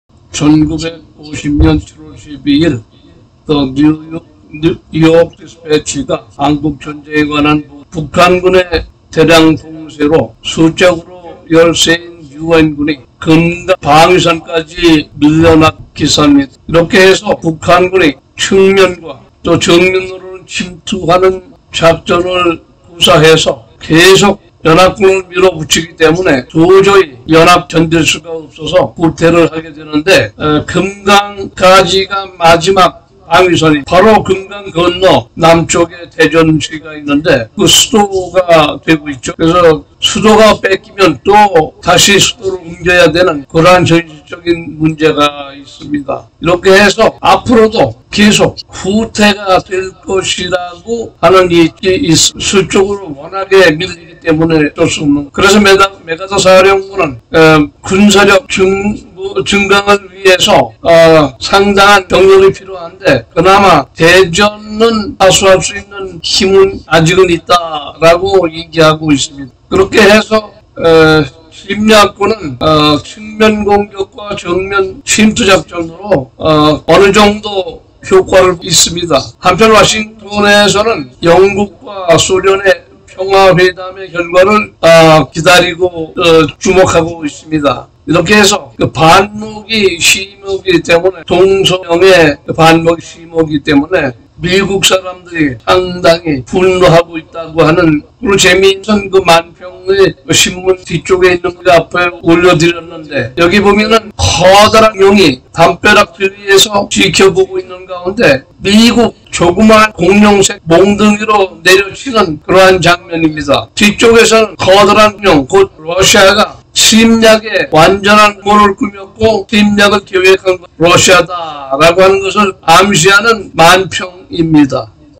Korean War Headline News of the York Dispatch, July 12, 1950
Series of Korean War news articles from June 25, 1950, to July 27, 1953